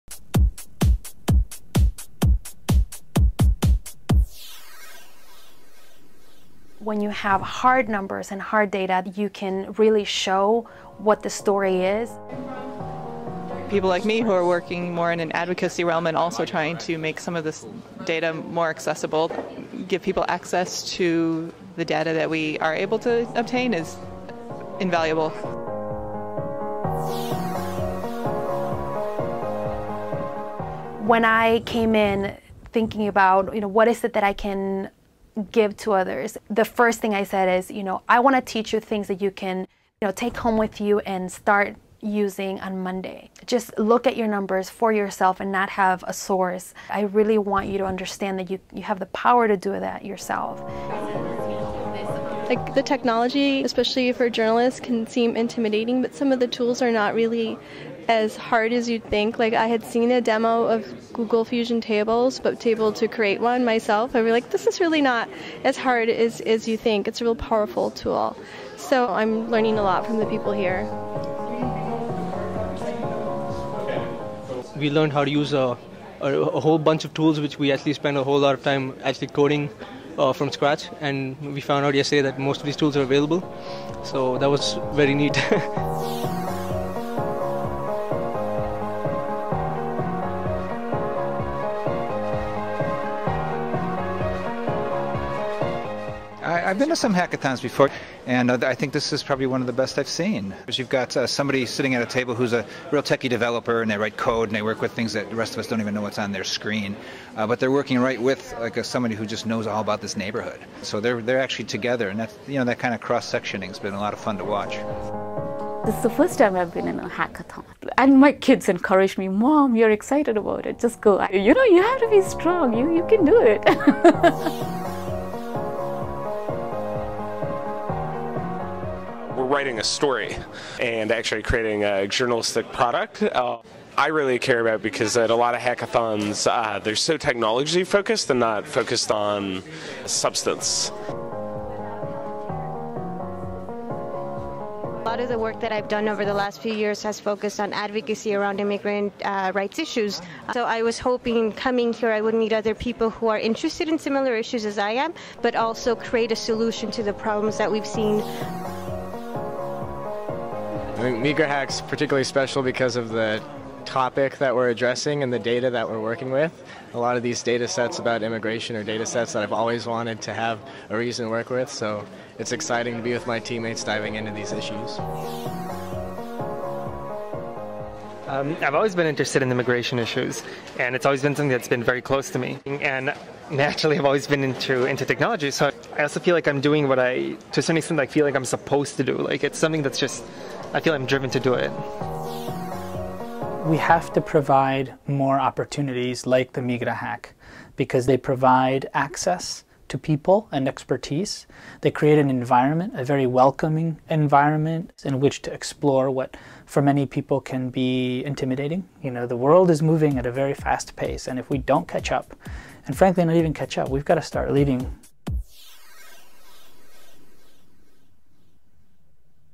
MIGRAHACK
Migrahack: Journalists, developers and community members combine forces.
The purpose of Chicago Migrahack is to foster the use of technology and open data to innovate information around immigration. In Spanish, the word "migra" refers to immigration.
We want to bring together our communities to create innovative platforms around one of the most debated topics today: Immigration
A lot of great projects came out of the MigraHack. All the projects are posted on MigraHack's website.
Below are some of them:
Finding Care, looked at undocumented immigrants left out of the Patient Protection and Affordable Care Act through the eyes of a young man whose immigration status made him ineligible to be placed on the transplant list for a kidney he desperately needed. It won the grand prize for best storytelling with data visualization.

Illinois in the Ice Age won in the best data visualization category.